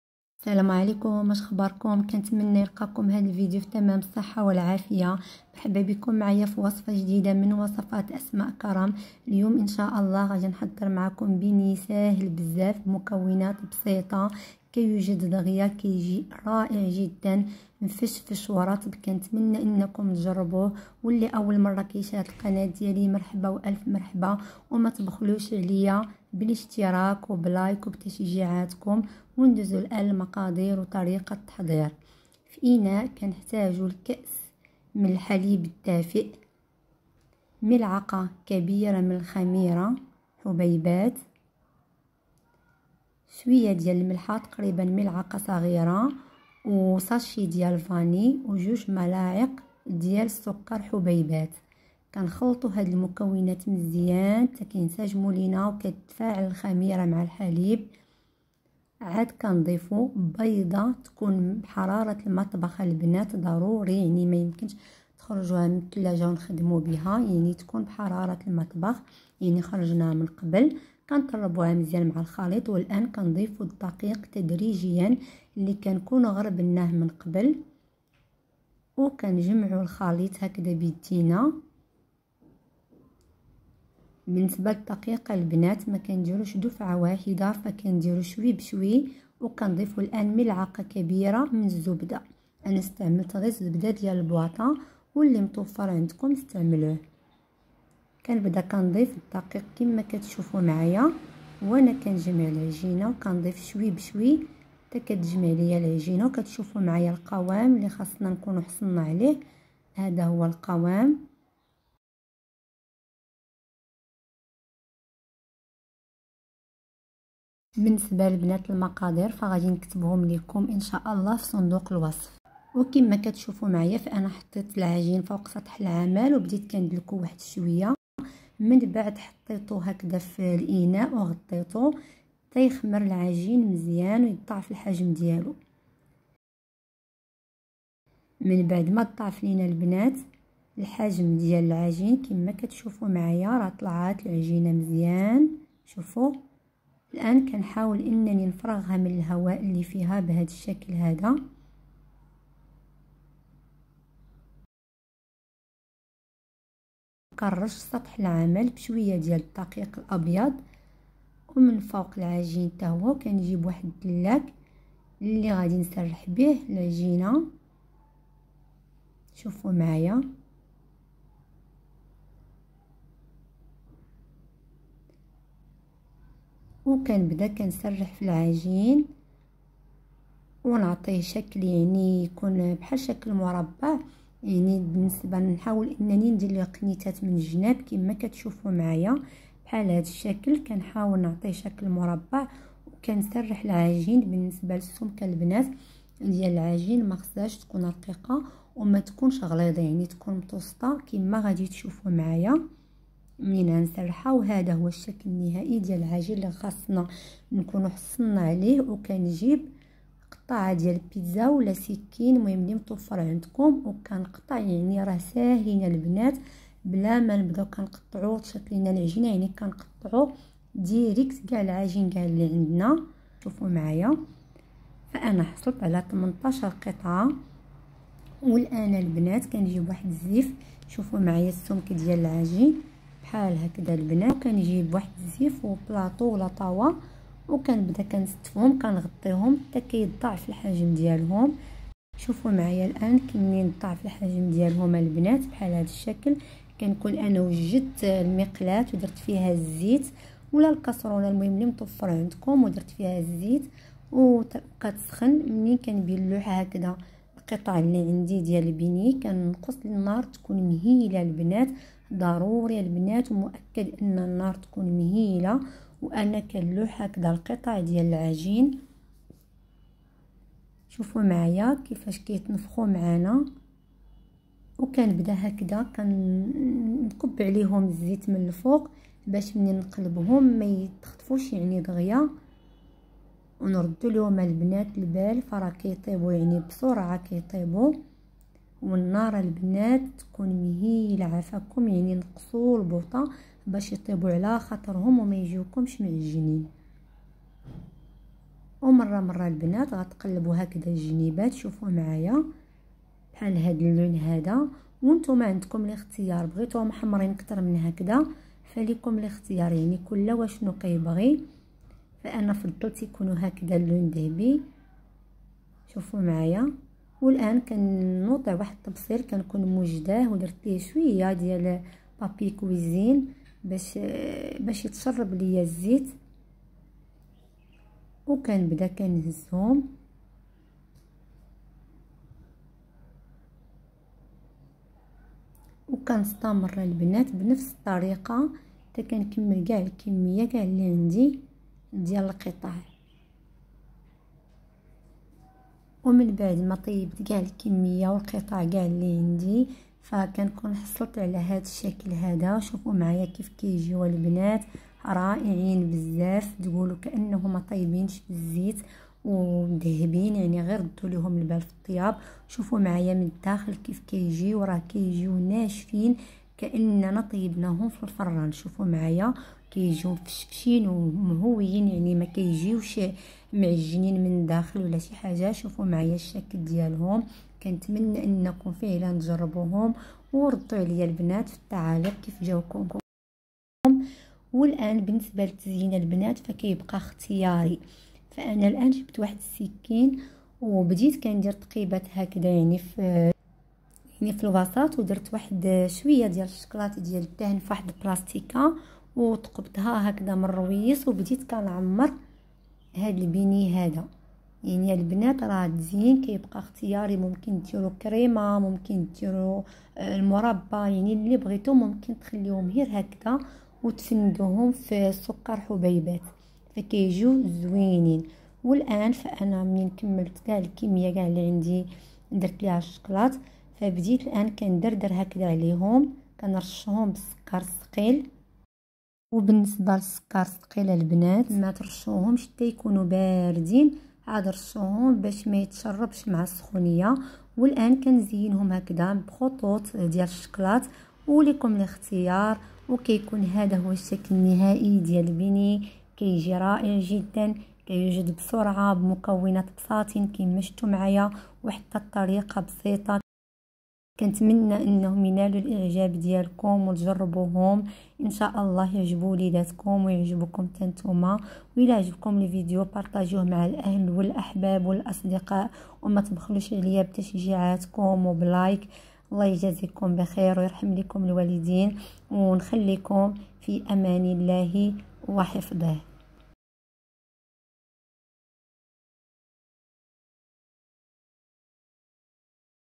وداعا للكسل في 10دقائق حضري بينيي ساهل بزاف بمكونات بسيطة ب بيضة واحدة ناجح 100% بدون مجهود ولا تعب
أسهل طريقة لتحضير بينيي ناجح اقتصادي ب بيضة واحدة فقط كيجي خفيف ومفشفش بدون مجه

المقادير:
200مل حليب دافئ
ملعقة كبيرة خميرة حبيبات
2ملاعق كبار سكر حبيبات
1ساشي فاني
1بيضة
500غرام دقيق أبيض